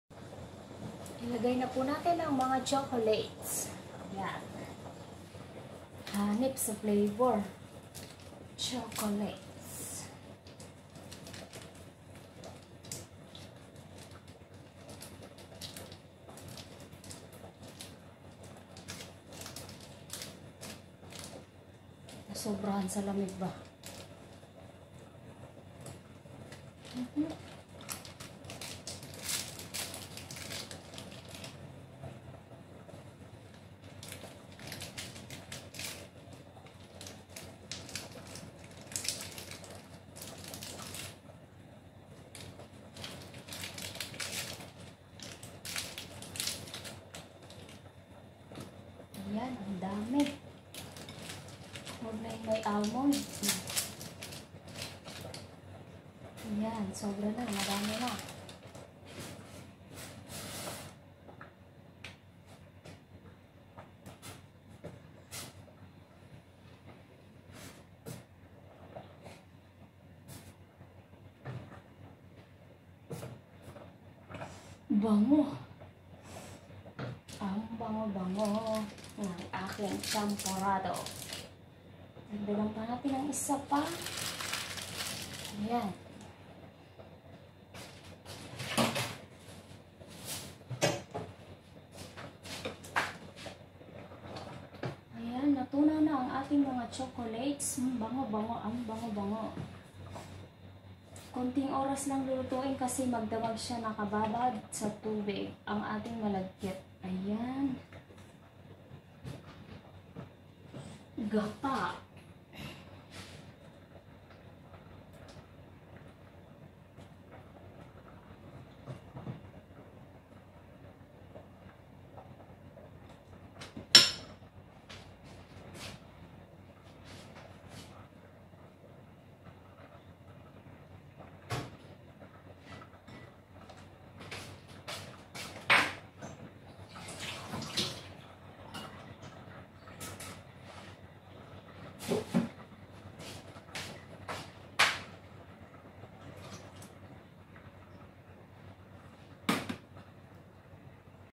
ChocolatesFlavoredChamporado|sallysoreta
2 can of coco milk
assorted chocolates
1 cup of glutinous rice
1/2 cup of purple rice
water..

wash the rice white and purple
boil together until soft
put the chocolates
add the cocomilk
add pinch of salt
boil until cocomilk is done.